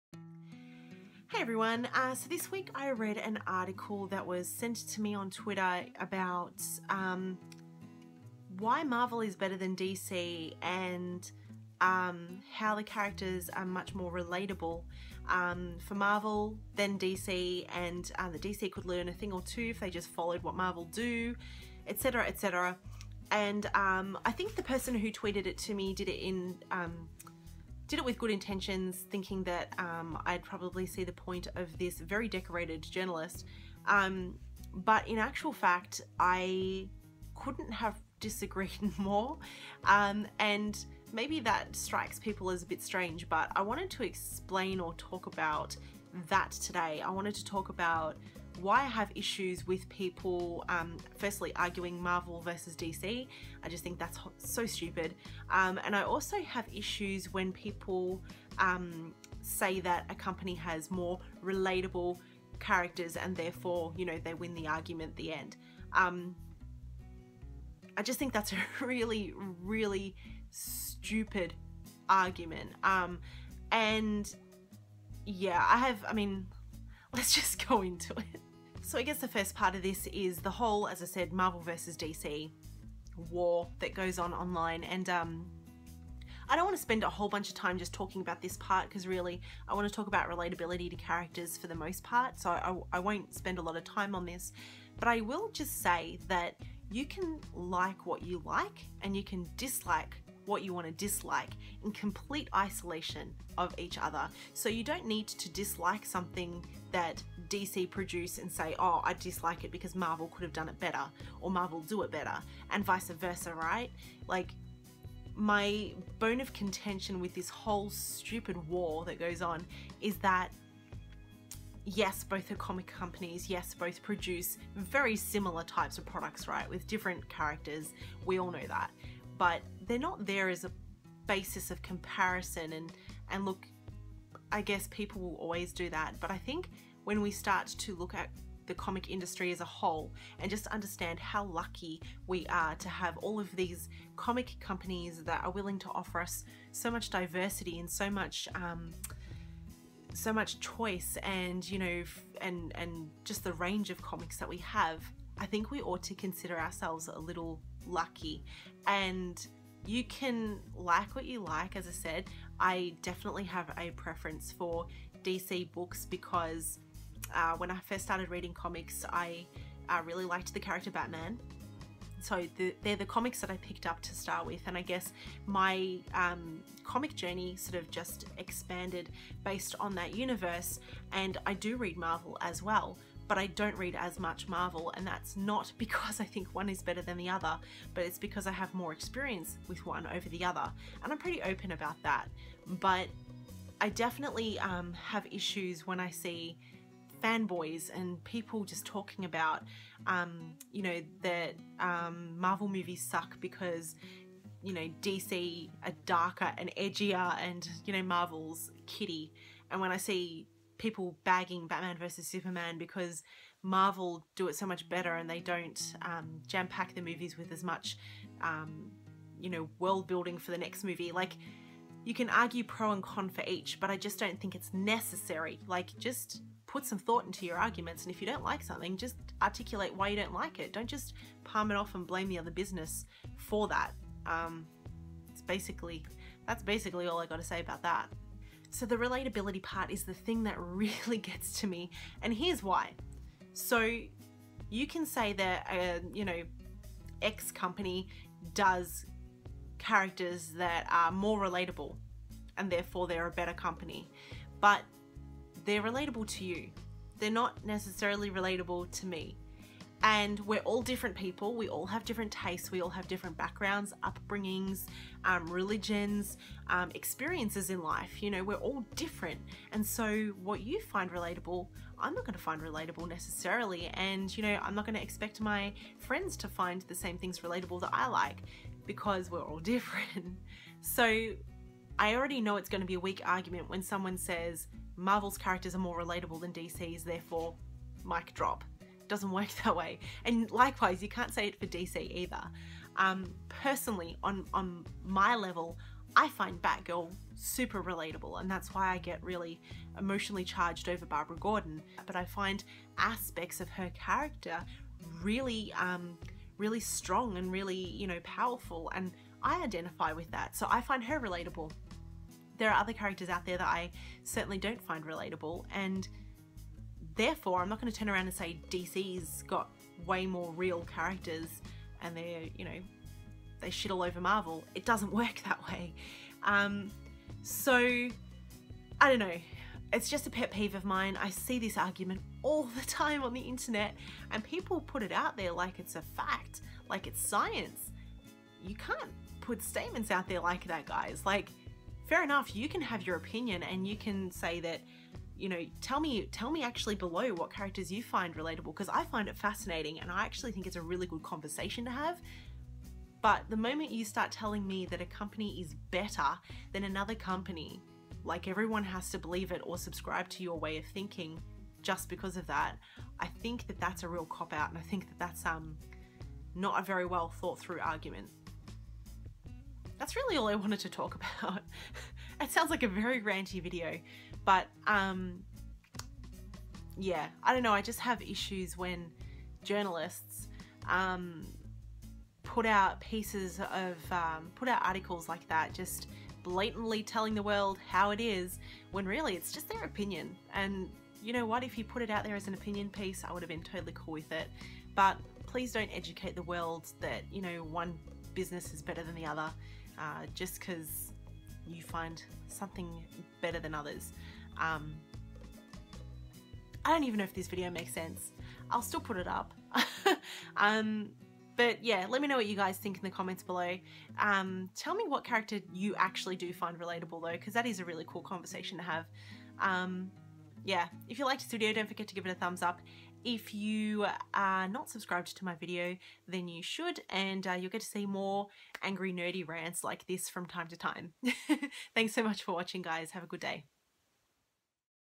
Relatable Comic Book Characters and Why The DC Vs Marvel Argument Is Dumb
DC and Marvel are both awesome comic companies, as are Image, Vertigo, Dark Horse, IDW and the others too. I don't love when people see the need to shit on something because they are too much of a fanboy for another publisher. We talk about that this week as well as people arguing online about one company being better than the other because "the characters are so much more relatable". That may be the case for you, but not for all.

COMIC BOOK WARS
RELATABLE COMIC CHARACTERS